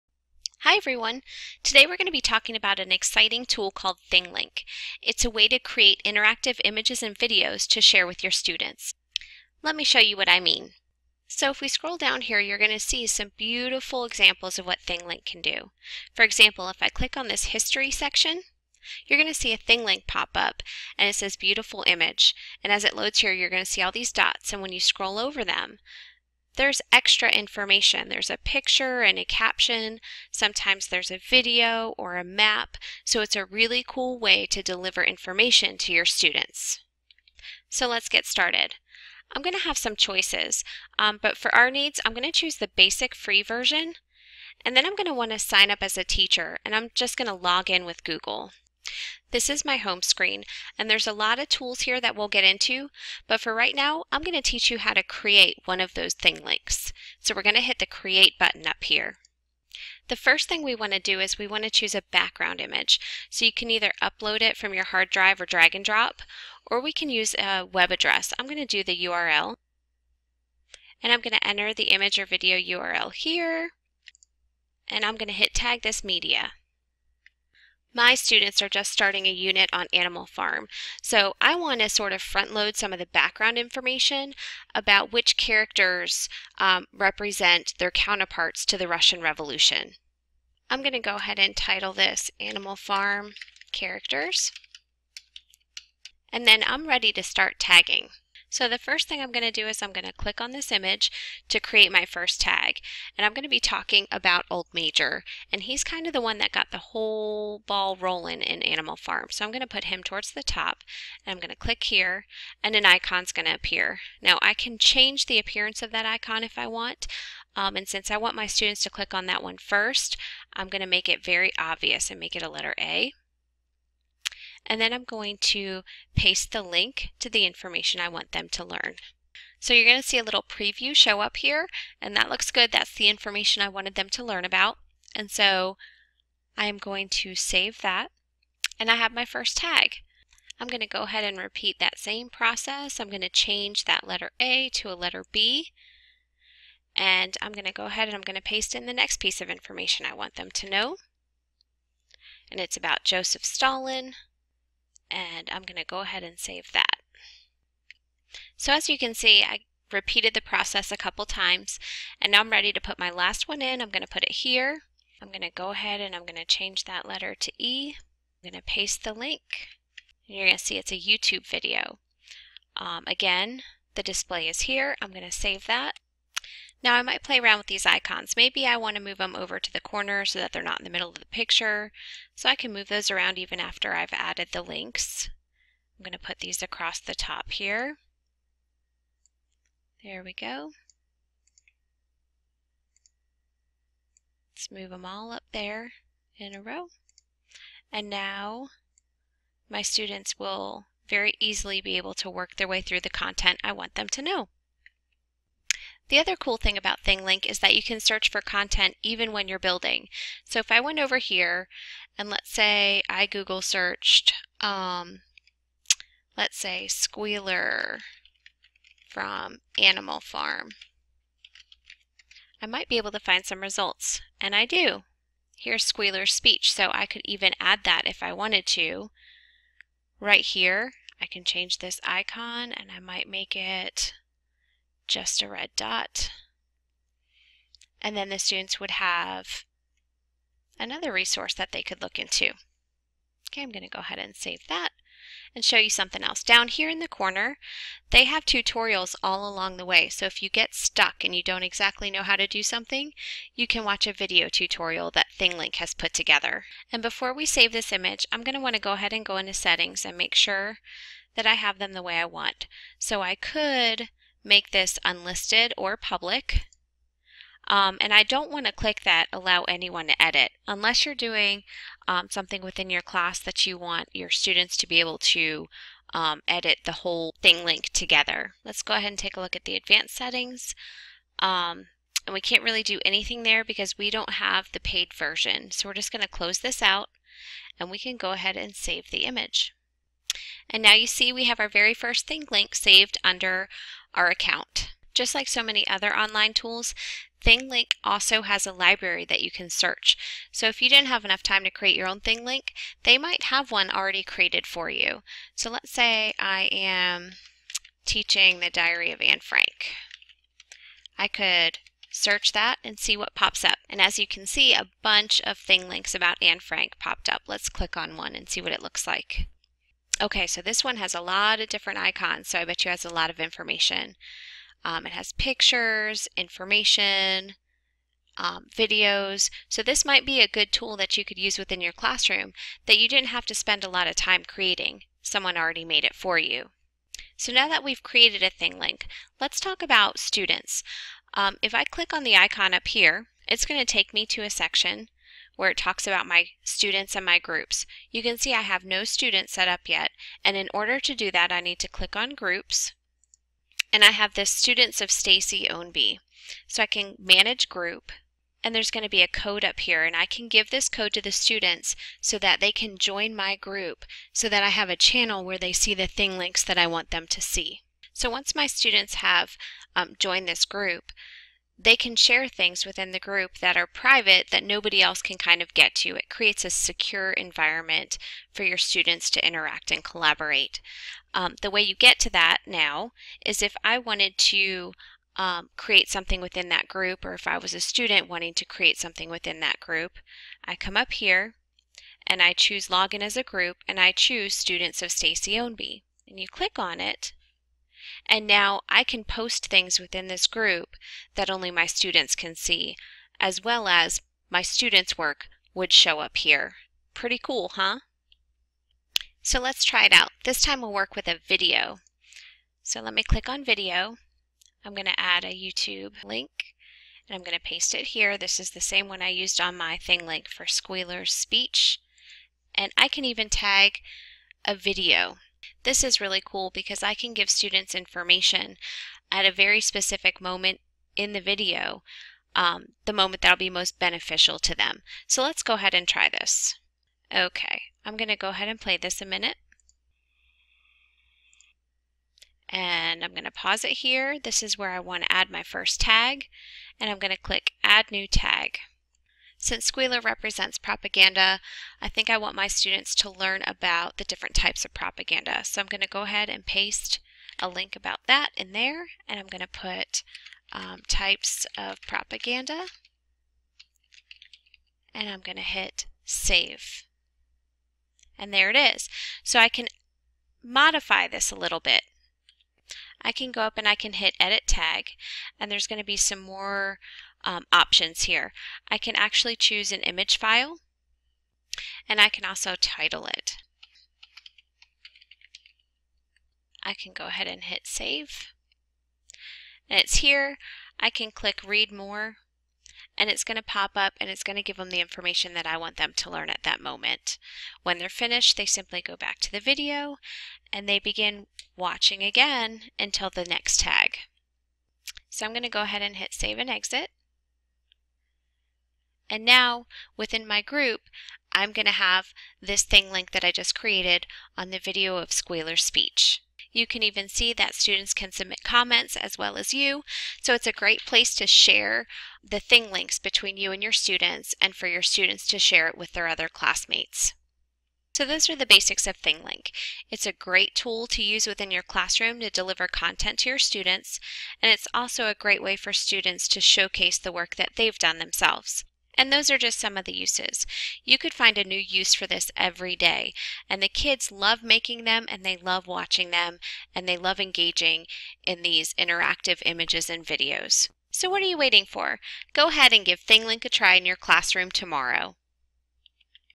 Thinglink Tutorial
Thinkglink is an online tool that allows teachers and students to create interactive photos and videos. You can layer different types of media including photos, videos, weblinks, and maps on a static image to make your pictures come alive. Students can explore a topic through the tags you place within a Thinglink to explore sources on a topic, complete research, or gain background information on a unit of study. Students can use it as a portfolio or interactive report. There are so many uses for Thinglink in the classroom and beyond. It's a great way to make a visual come to life and engage students in learning.